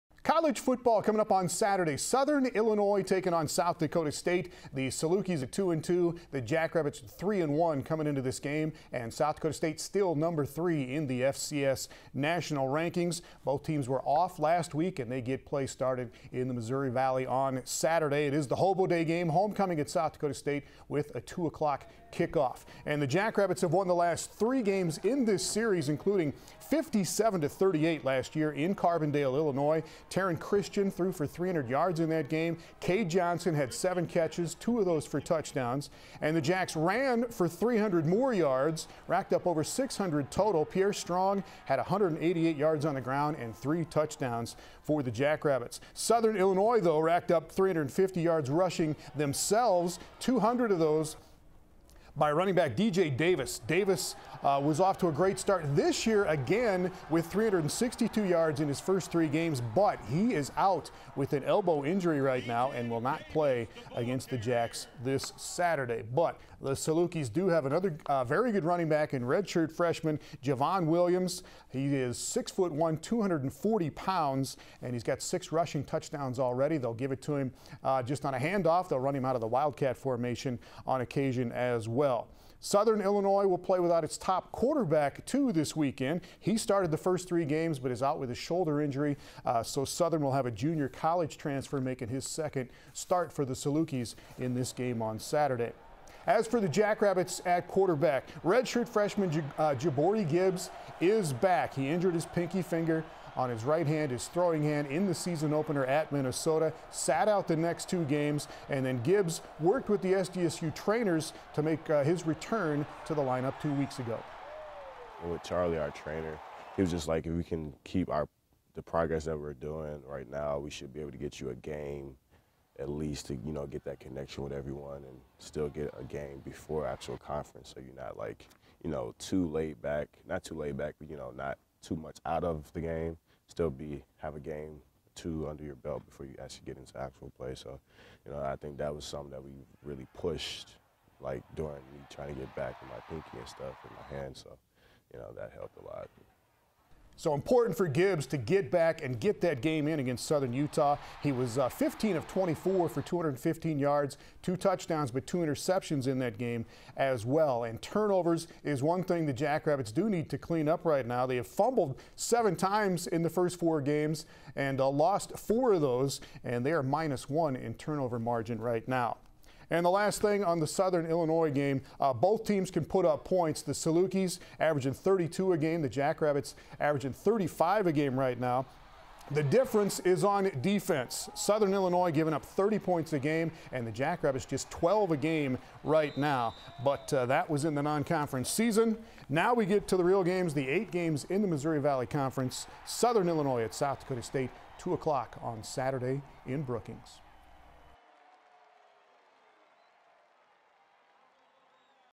Southern Illinois SDSU football Preview | 10/4/19
South Dakota State plays host to Southern Illinois on Saturday to open play in the Missouri Valley Football Conference. The Jackrabbits and Salukis combined for 95 points last year in Carbondale so both teams are looking to hold it down on D this week. SDSU redshirt-freshman QB J’Bore Gibbs talks about his return to the lineup after missing two games with a hand injury.  Tom Nieman has the lowdown on the Hobo Day showdown in Brookings.